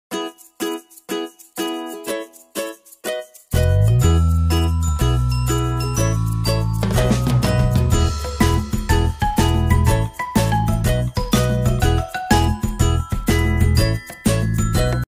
JR武蔵野線 吉川美南駅 開業10周年記念㊗️新幹線🚄がやって来た！♪😆
吉川美南駅開業10周年記念にミニ新幹線がやって来ました♪